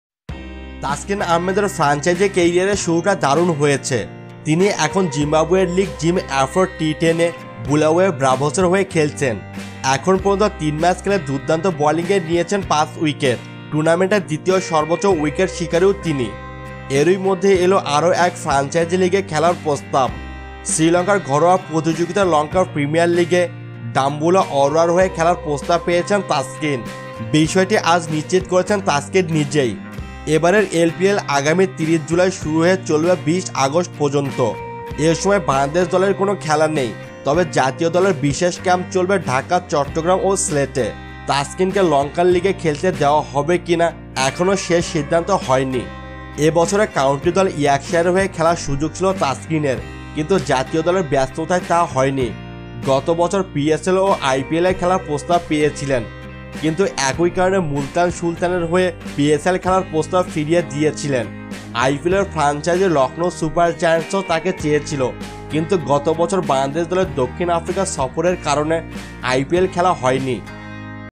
একের পর এক দুর্দান্ত পারফরম্যান্সে নজর কেড়ে এবার শ্রীলঙ্কা প্রিমিয়ার লিগে দল পেলেন তাসকিন! Taskin
Zim Afro T10 League || Taskin Ahmed || Lanka Premier League || Bangladesh National Cricket team || Bulawayo Braves